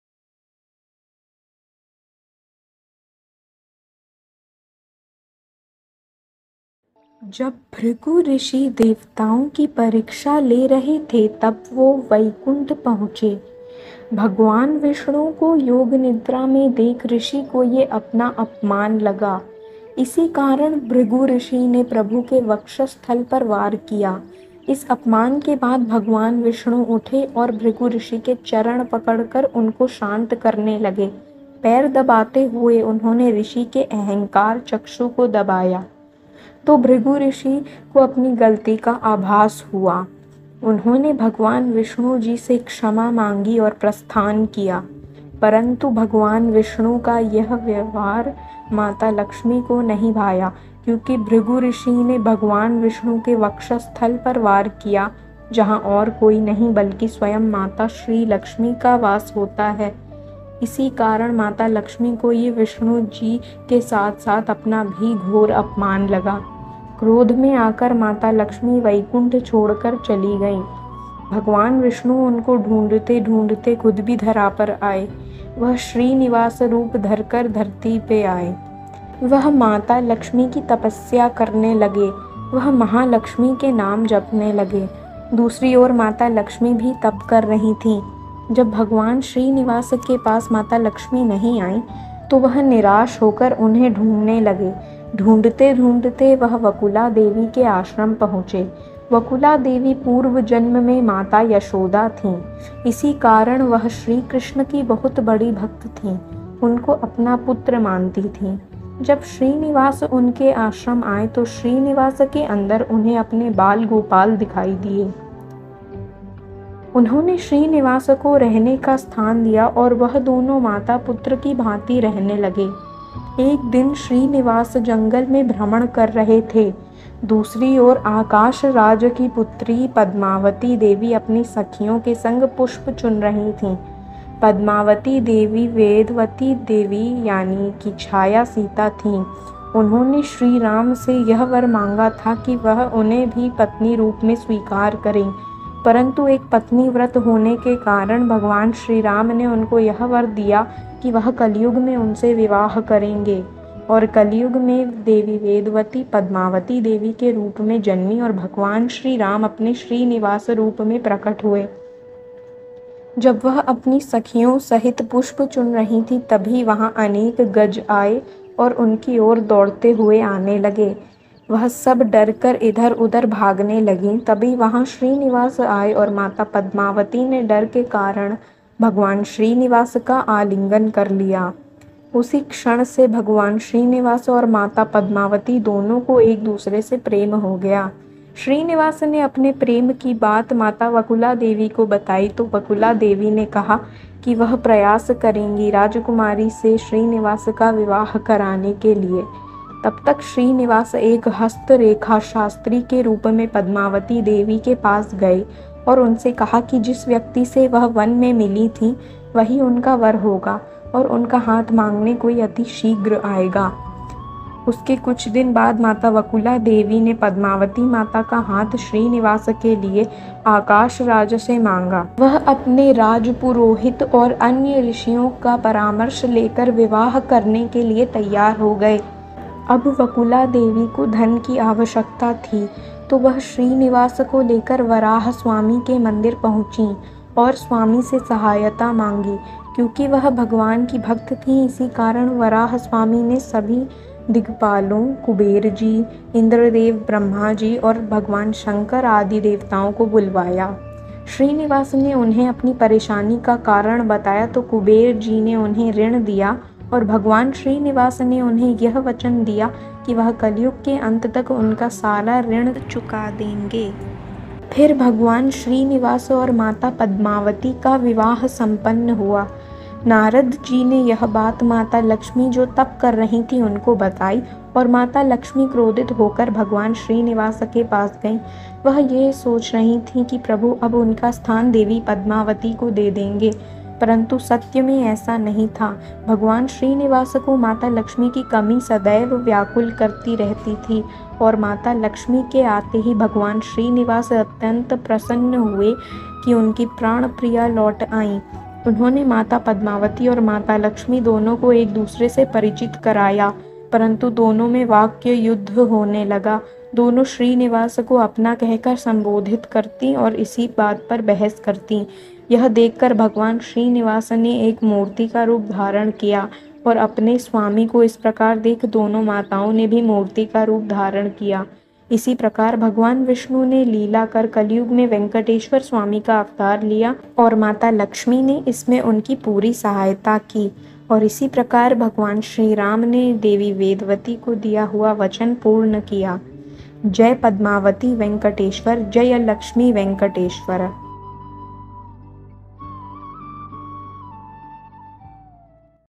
भगवान् श्रीनिवास की कथा | Story of bhagavan Shrinivasa
There are many versions of Shri Venkateshwara Swami's avatar story . This is one of it. Jaya Padmavati Lakshmi Venkateshwara